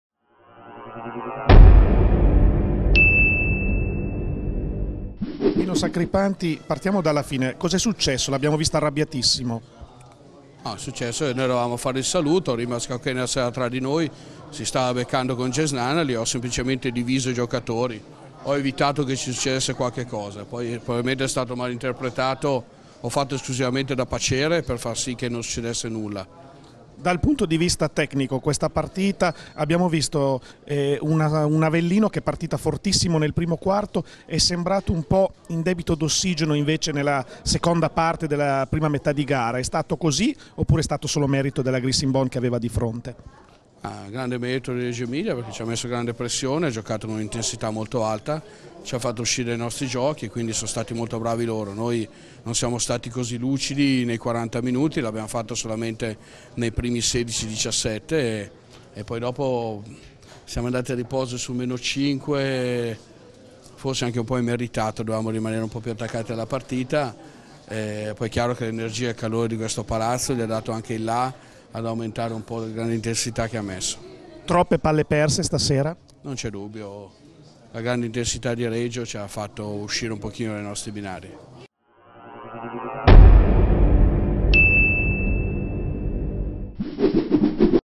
Sacripanti: "Ho fatto da paciere tra Nunnally e Kaukenas"
Il commento a fine gara del coach della Sidigas Scandone Avellino